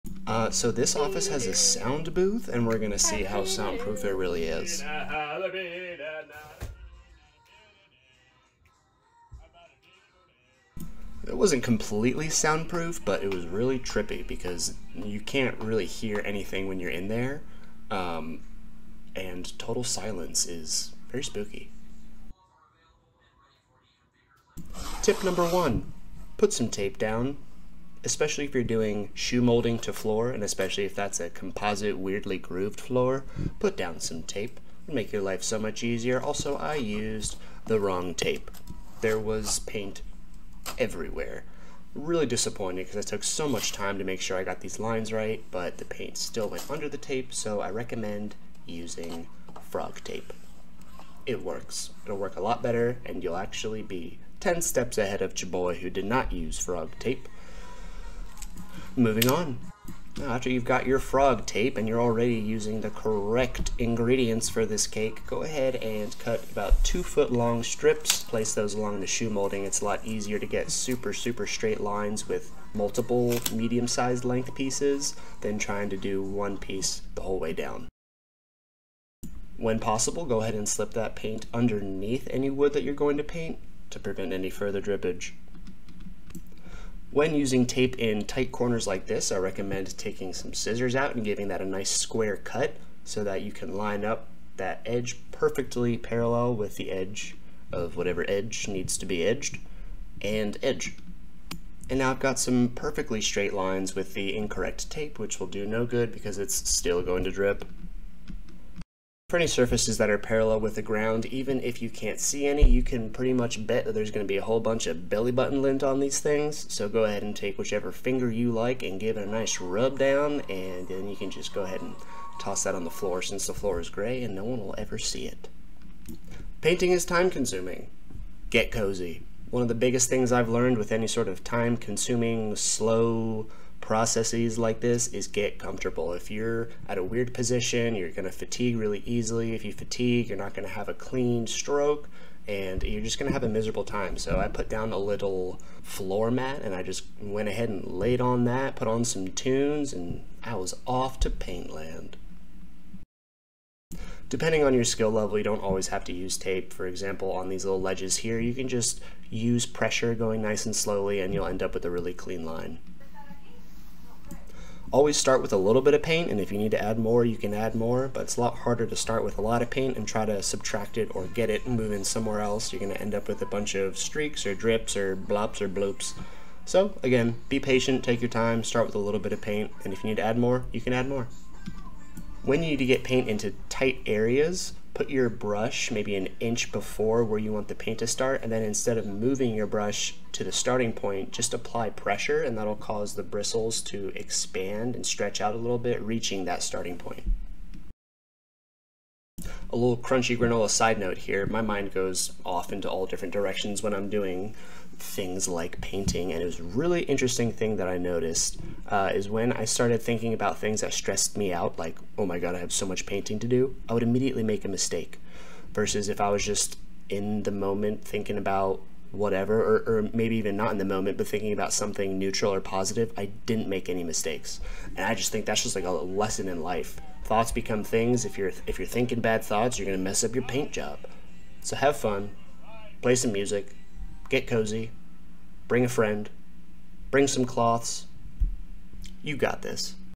How to paint inside!!
Ill go over tips as well as mistakes I've made so you don't have to! Painting interior may seem daunting but with some practice and tips it can be very therapeutic!